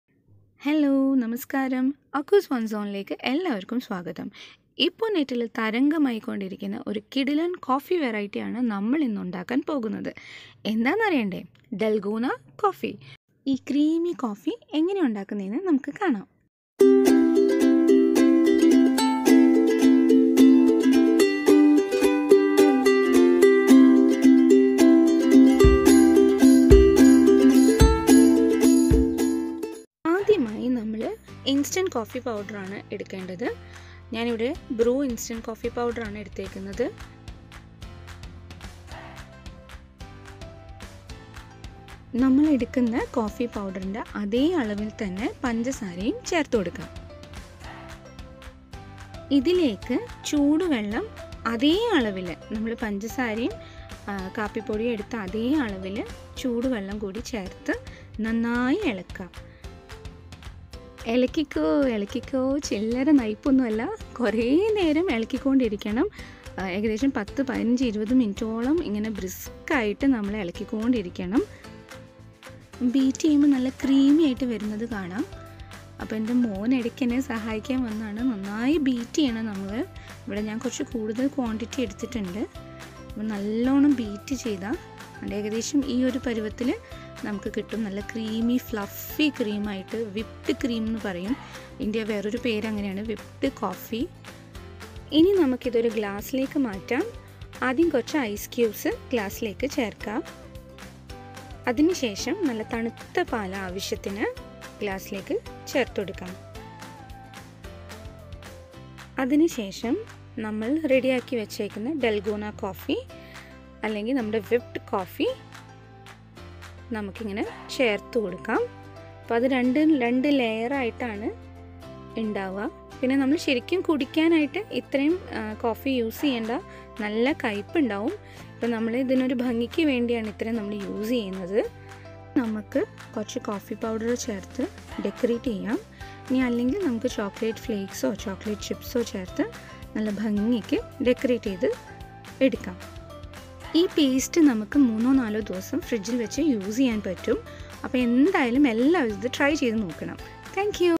Dalgona Coffee Recipe | ഡൽഗോന കോഫി-ടേസ്റ്റിലും ലുക്കിലും ഇവൻ രാജാവ് തന്നെ!
Sharing the recipe and making tips of the now trending DALGONA COFFEE!!

Watch the video and try it yourself!!
Enjoy a creamy whipped coffee at home!!Yum!!

Dont forget to serve it chilled for best taste!